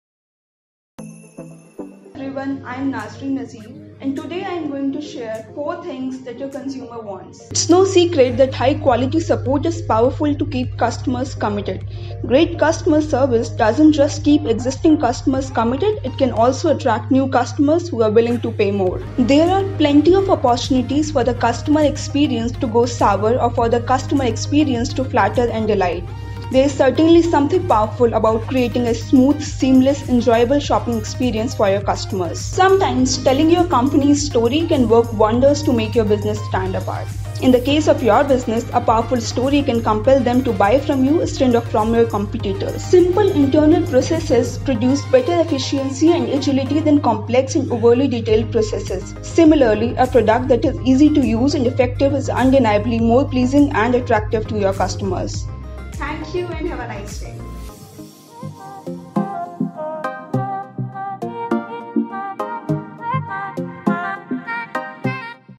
Things to Know For July 3: News that Entrepreneurs Can Use
Our Features Writer, Nazreen Nazir, gives a sneak peek into the important tips for entrepreneurs.  Here are 4 things that your consumer wants from you